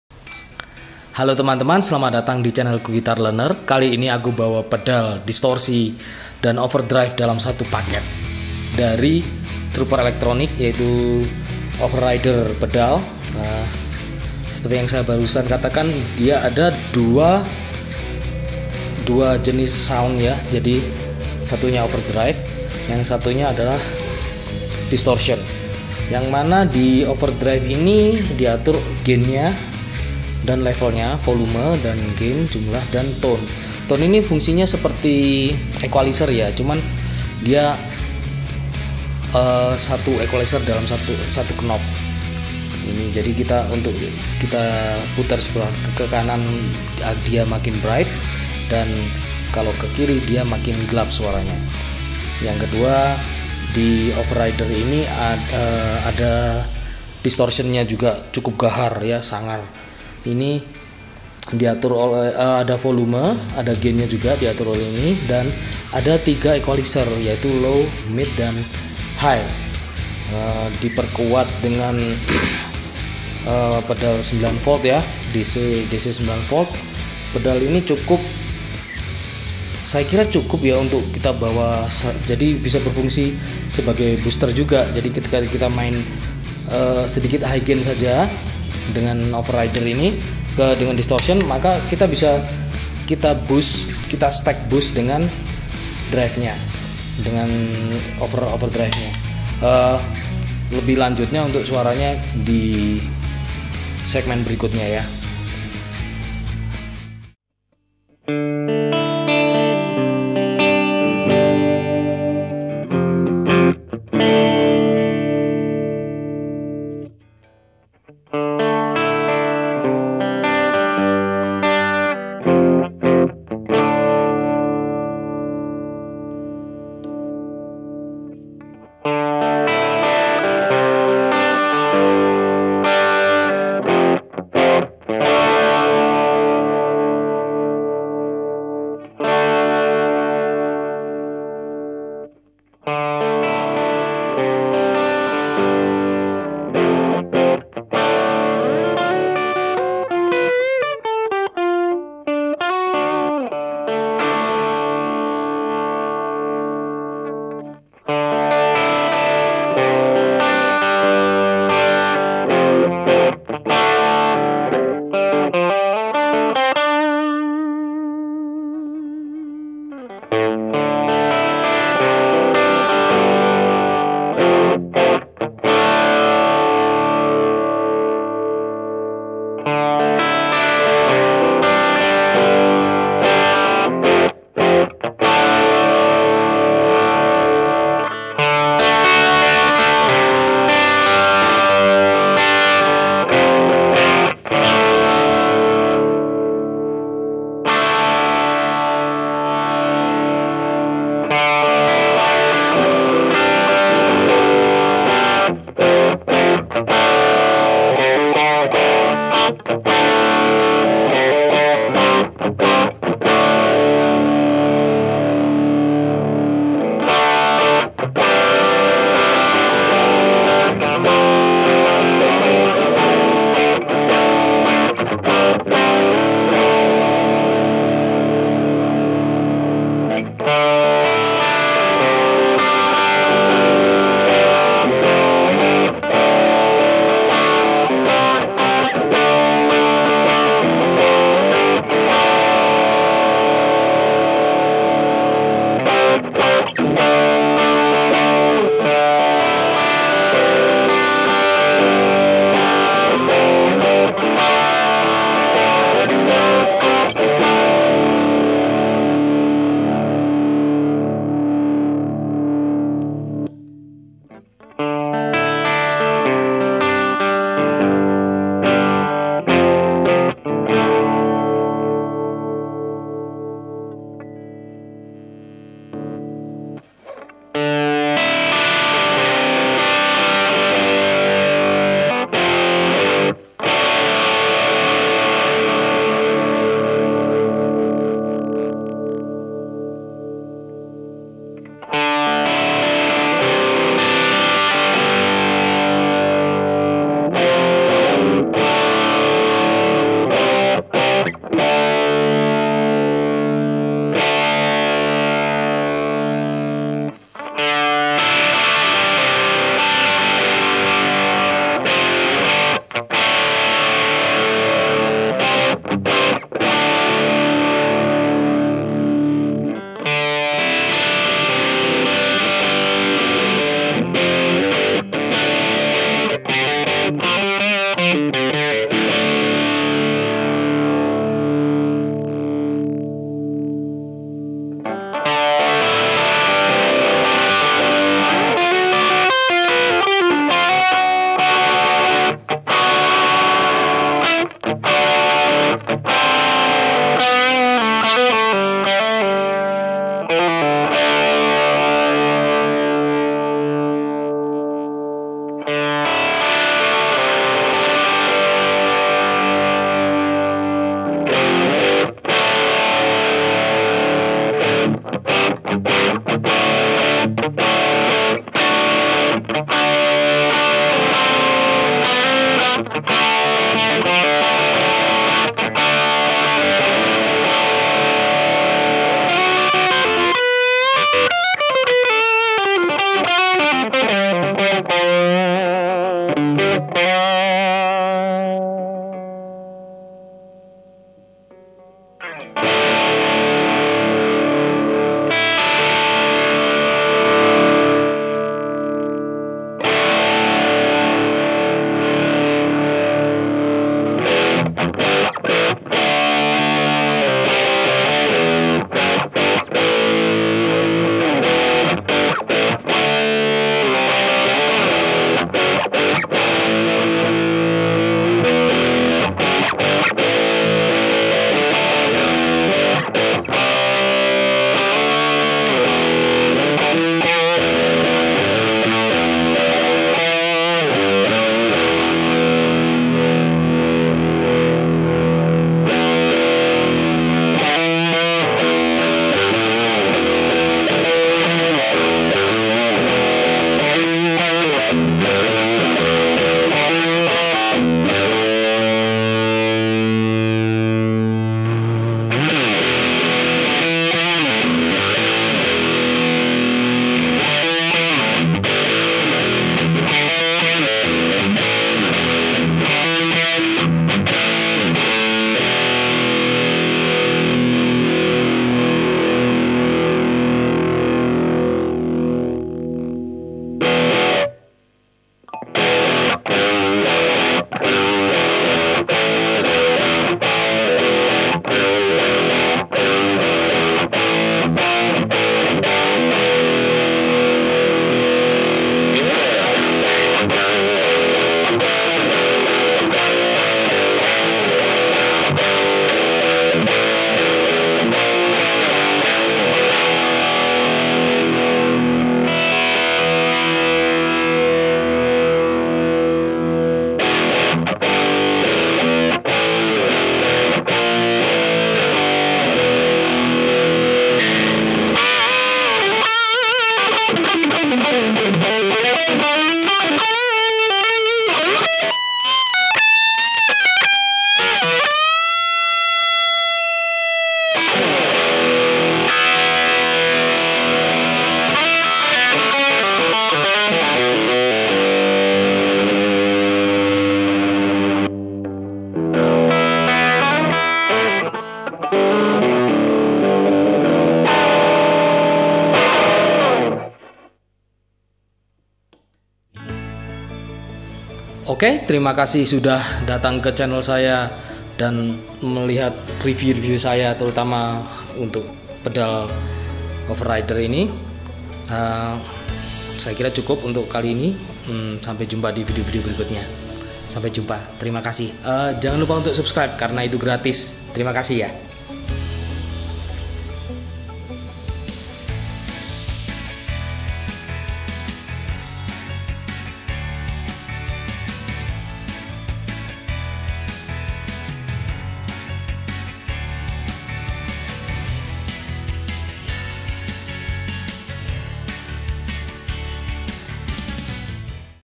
OverRider distortion overdrive pedal By Trooper Electronic
pedal lokal made in Indonesia yang gahar
Pedal ini memiliki jangkauan tone mulai dari Vintage hingga modern metal. ada 2 jenis Channel Distortion (over) dan Overdrive (rider) stacked